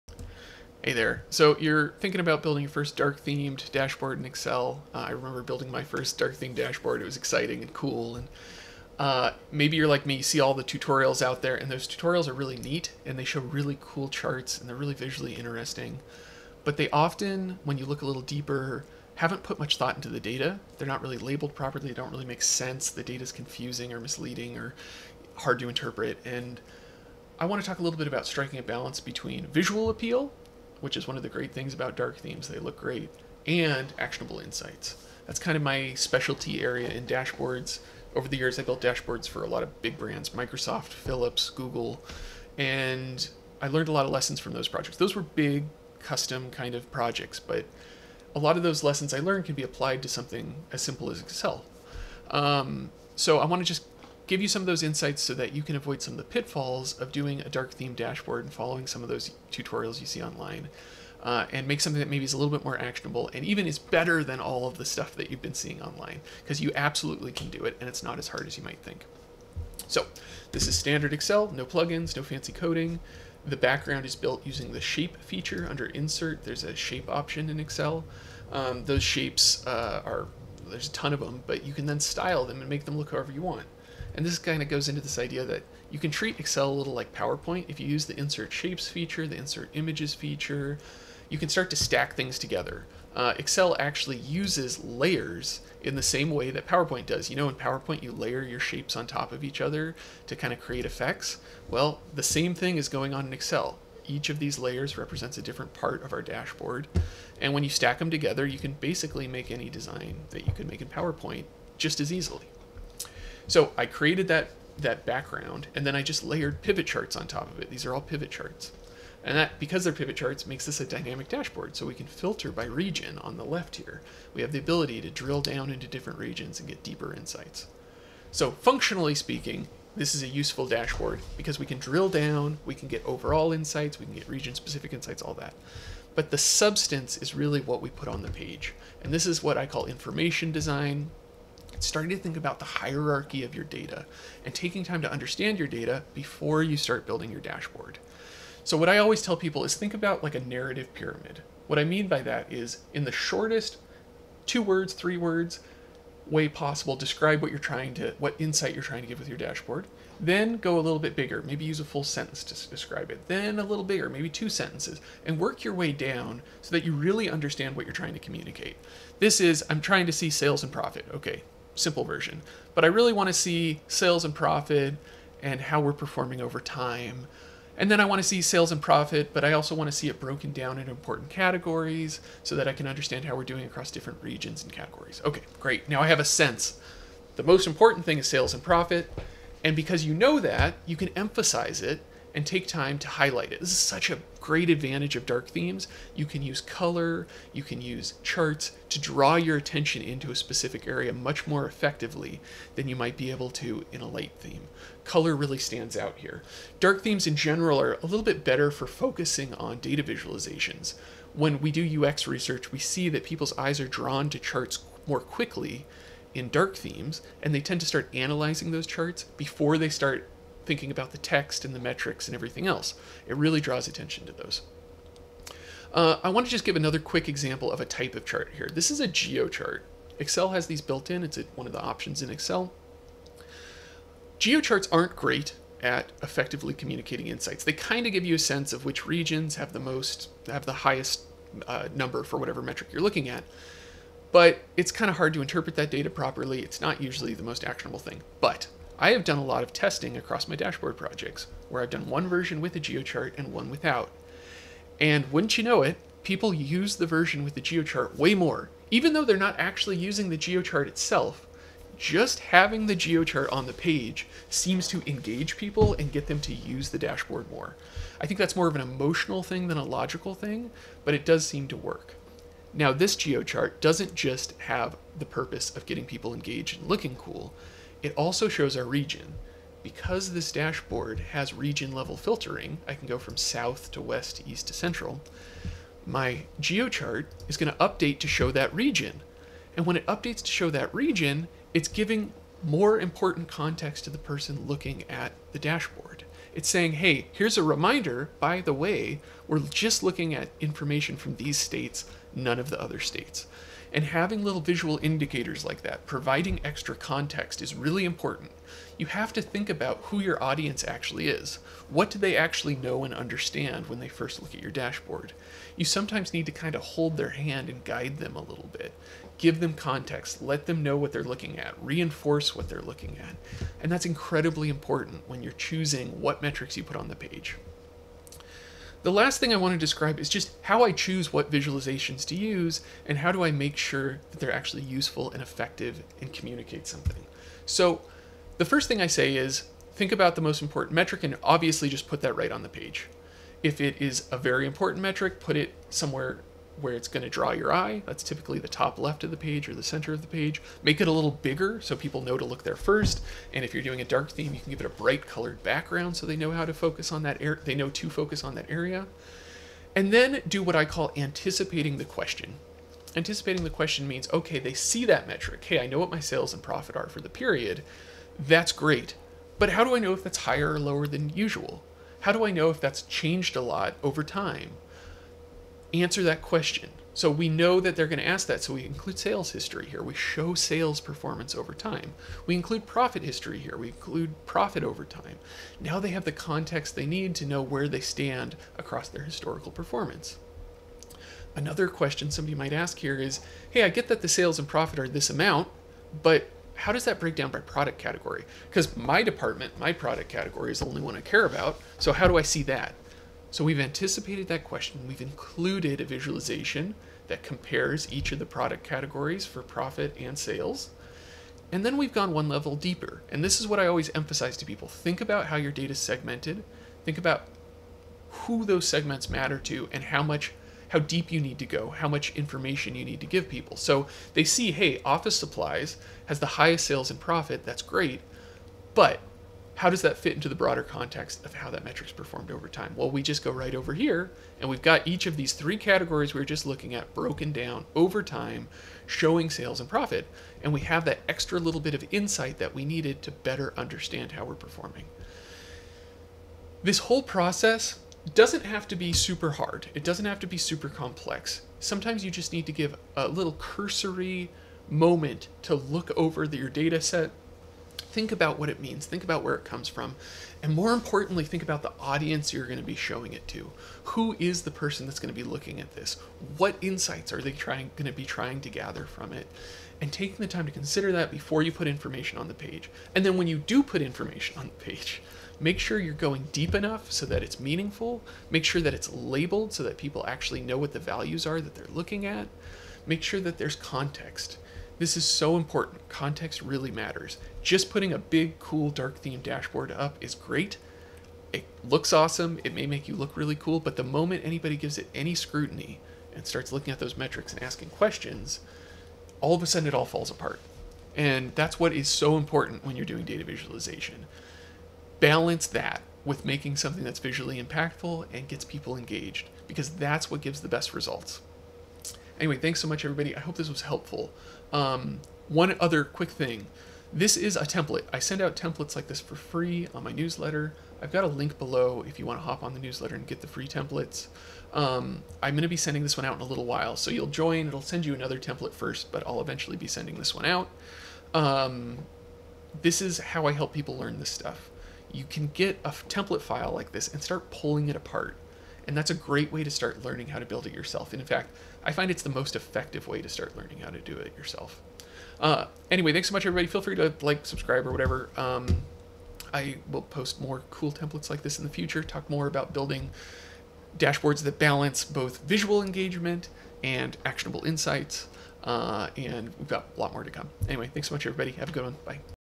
Style over substance in your Excel dashboard design (includes sample files)
A lot of tutorials on youtube show you how to build dark dashboards that look great but don't communicate effectively with data. It's important to strike a balance between visual style and actionable insights. This is how I make dark themed Excel dashboards that look great and help people gather useful insights from their data.